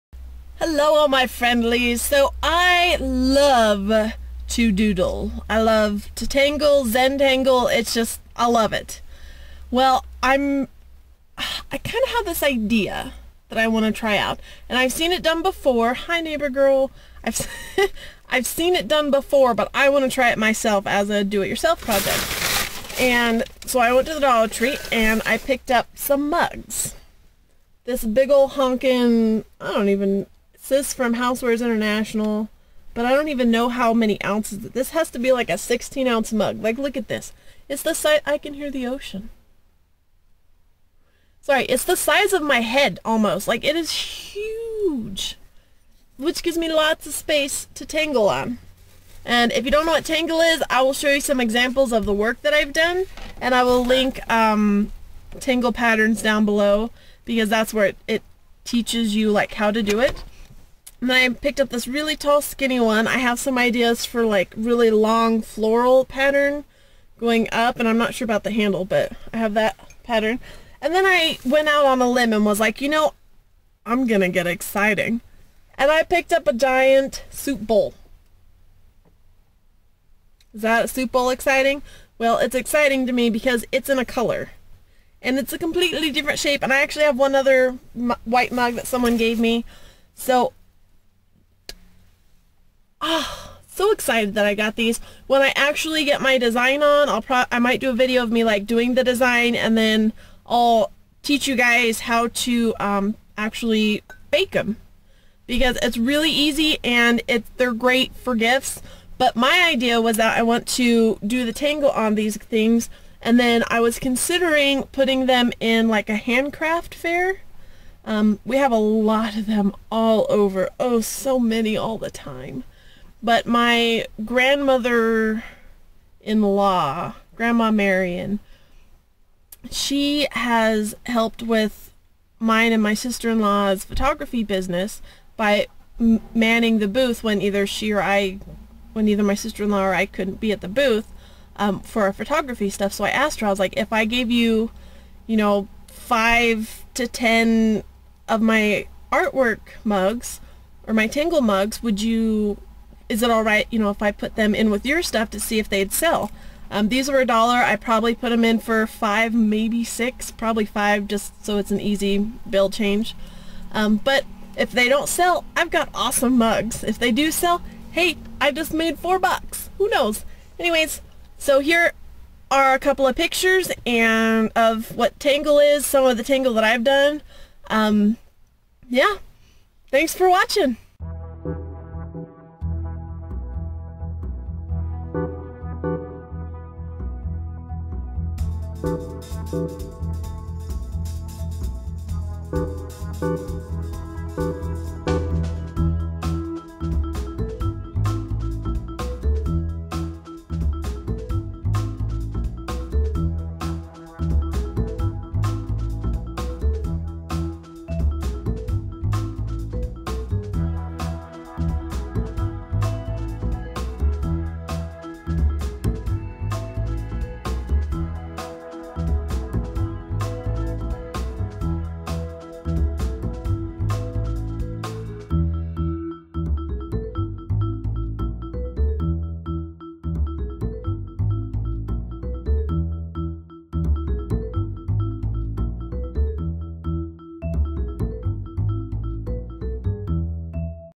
Dollar Tree Haul Part 2 || Zentangle Canvases!
I have a project in mind, and here are the canvases I chose to work with!!!